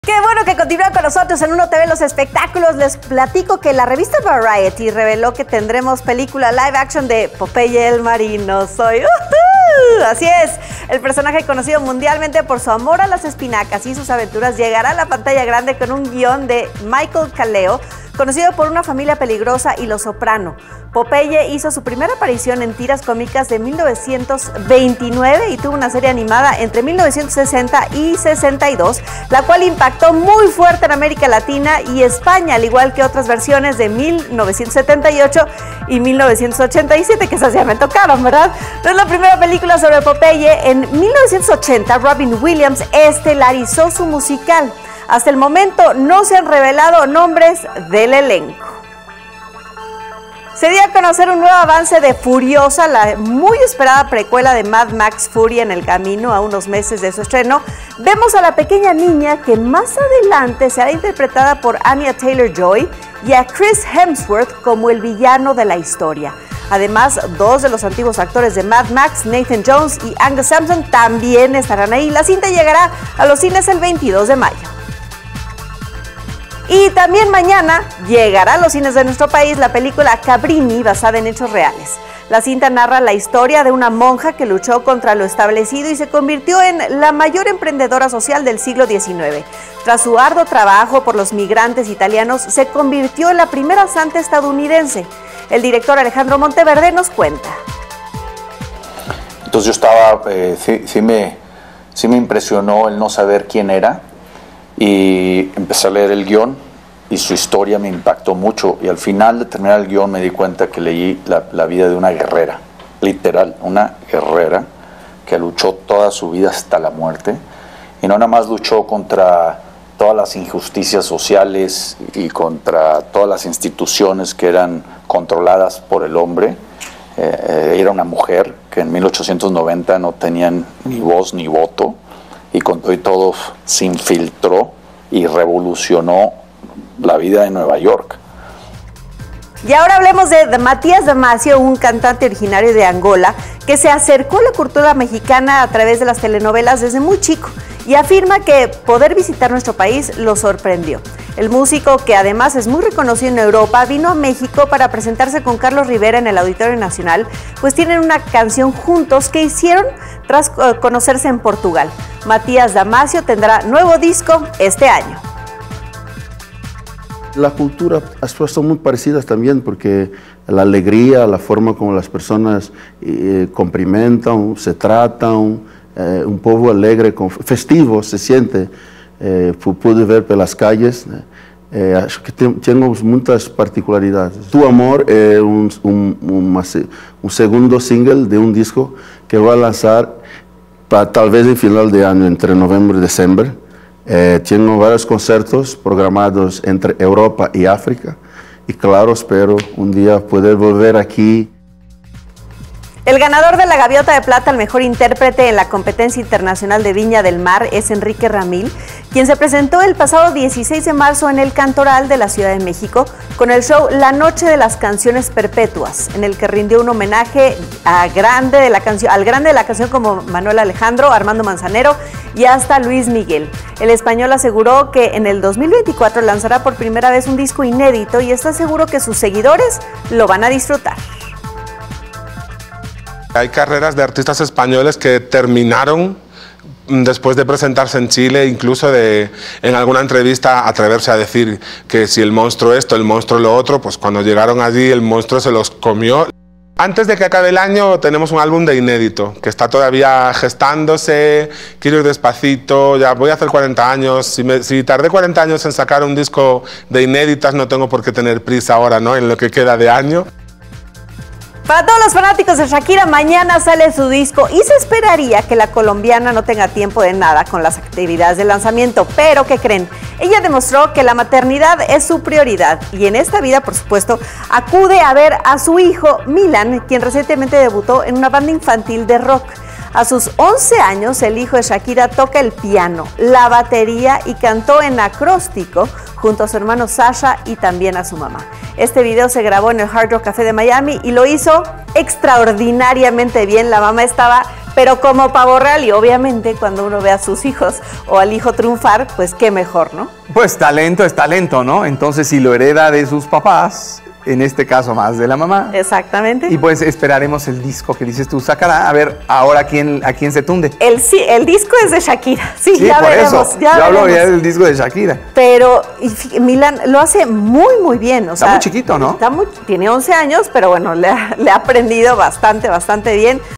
Popeye tendrá su live-action; se estrena Cabrini
La revista Variety confirmó que Popeye tendrá su live-action, aunque no dio fecha de estreno; mañana se estrena "Cabrini" en cines. El hijo de Shakira debutó como baterista en Miami.

00:00 Popeye tendrá su propio live-action
00:40 Nuevas imágenes de Furiosa
01:08 Cabrini llega a las salas de cine
01:52 Matías Damasio cantó en el Auditorio Nacional
02:50 Enrique Ramil lanzará disco inédito
03:53 Shakira se da tiempo para ir a show de su hijo